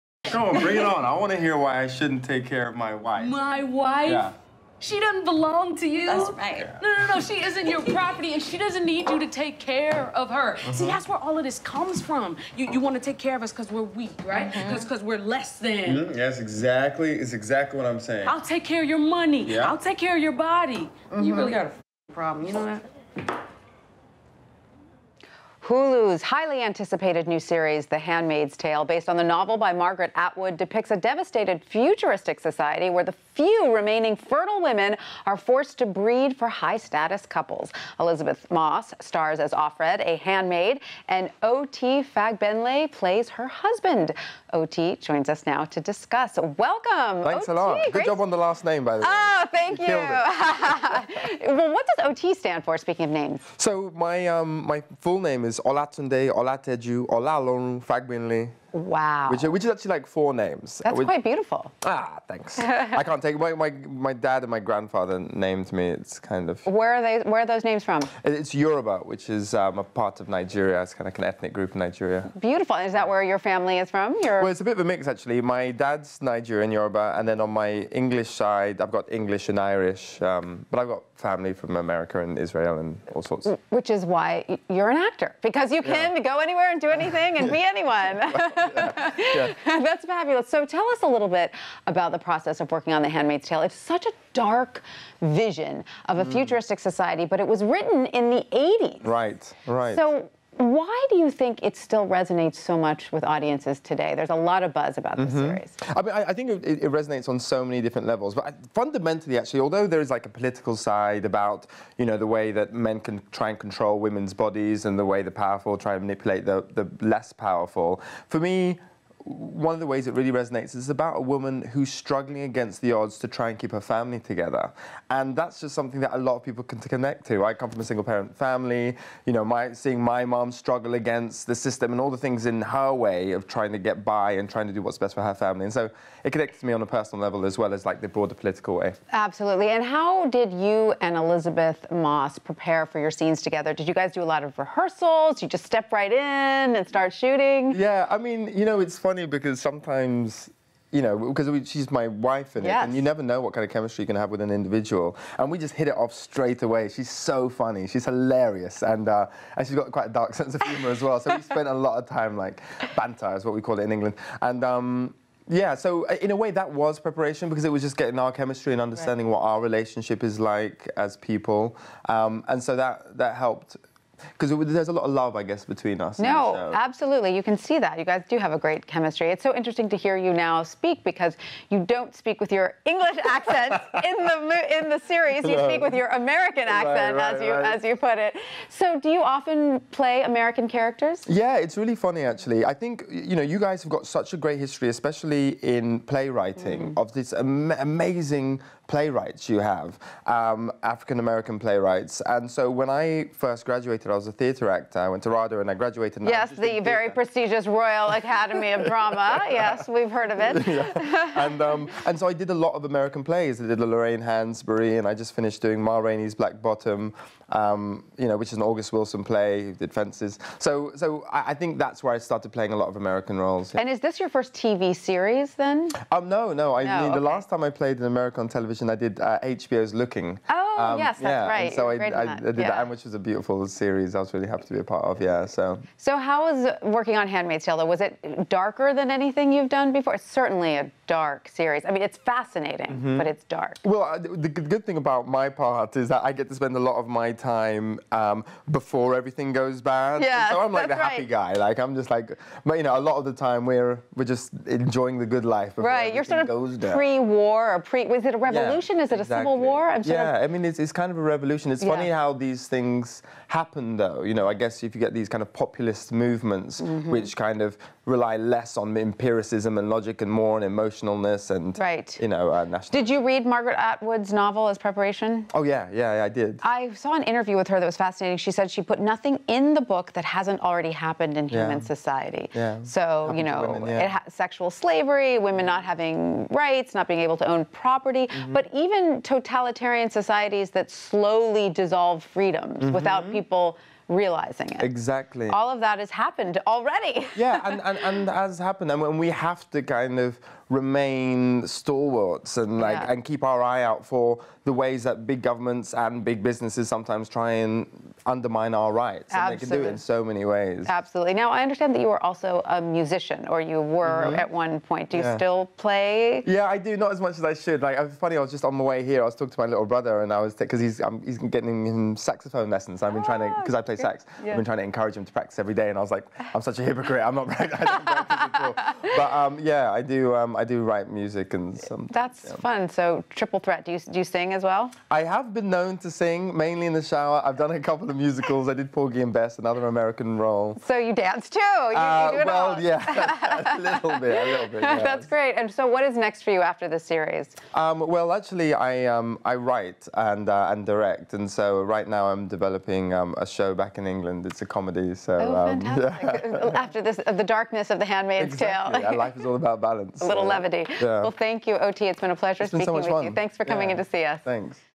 'The Handmaid's Tale' Star O.T. Fagbenle
O.T. Fagbenle, star of Hulu's original series "The Handmaid's Tale," joins Lunch Break's Tanya Rivero to discuss working with co-star Elizabeth Moss and bringing to life Margaret Atwood's dystopian classic.